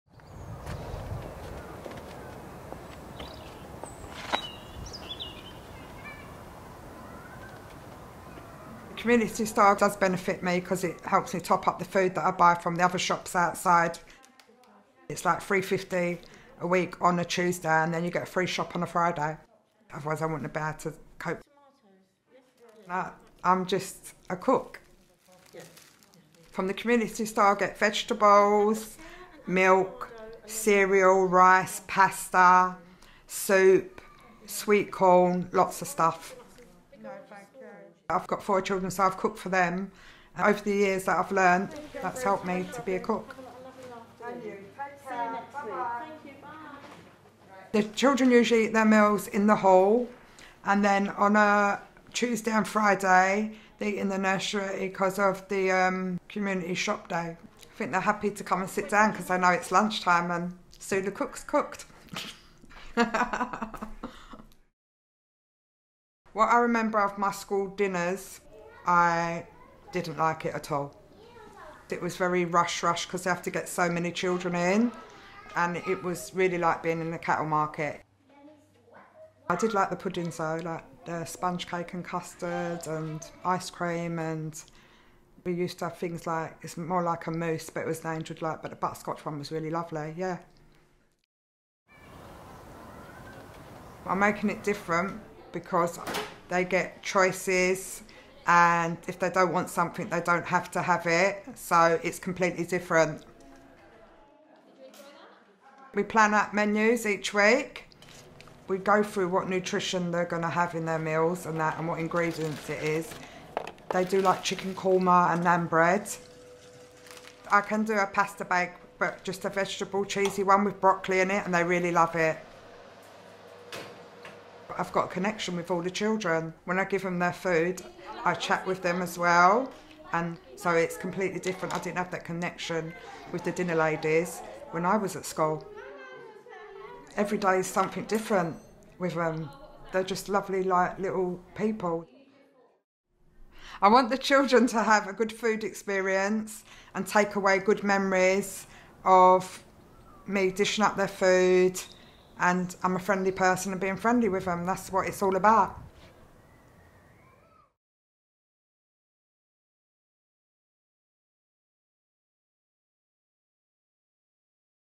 Evelyn Community Centre: Sue the cook
We accompany Sue on one of her shopping trips to the local community store within the Evelyn Community Centre in Deptford. It is also where Sue works part-time as a cook preparing meals for the children attending the nursery.
The  award winning community food store, run by Lewisham Homes residents and other volunteers, has become an important asset for many families who live locally.
It aims to reduce food poverty by allowing residents to pay £3.50 per week for £30 worth of food.